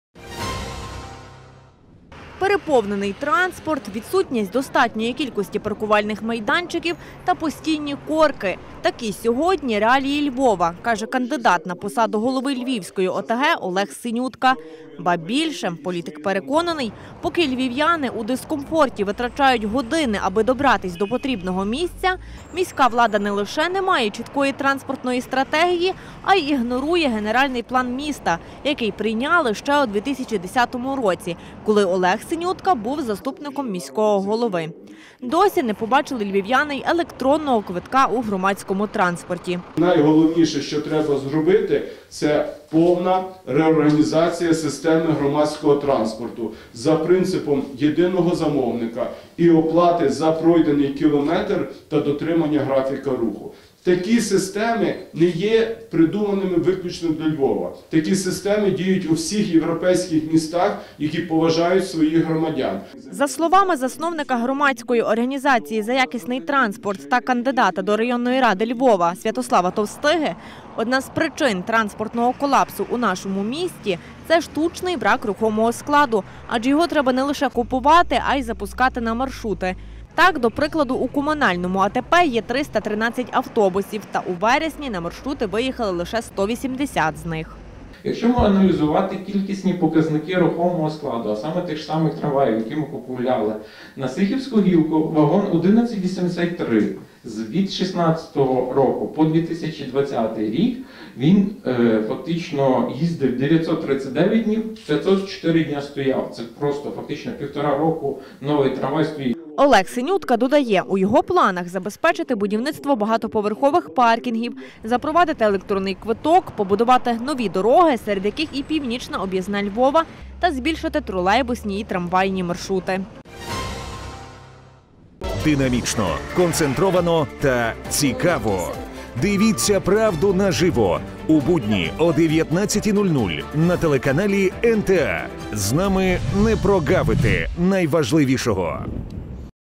Як вирішити транспортні проблеми Львова – розповів Олег Синютка
Переповнений транспорт, відсутність достатньої кількості паркувальних майданчиків та постійні корки - такі сьогодні реалії Львова, каже кандидат на посаду голови  Львівської ОТГ Олег Синютка. Ба більше, політик переконаний -  поки львів'яни у дискомфорті витрачають години, аби добратись до потрібного місця, міська влада не лише не має чіткої транспортної стратегії, а й ігнорує Генеральний план міста, який прийняли ще у 2010 році, коли Олег Синютка  був заступником міського голови. Досі не побачили
львів'яни й електронного квитка у громадському транспорті.